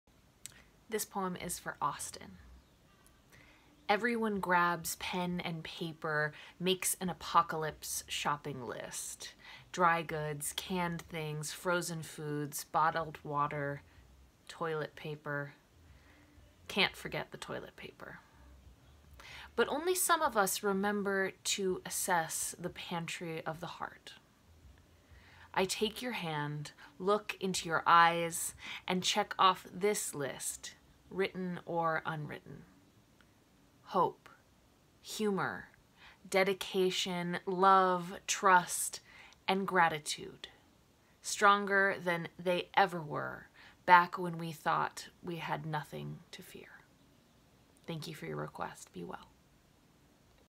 Poems for a World on Pause: dedicated to Austin
everyone grabs a pen and paper,
makes an apocalypse shopping list:
dry goods
canned things
frozen foods
bottled water
toilet paper
can’t forget the toilet paper

but only some of us
remember to assess
the pantry of the heart

i take your hand
look into your eyes
and check off this list, written or unwritten:
hope
humor
dedication
love
trust
and gratitude
stronger than they ever were
back when we thought we had nothing to fear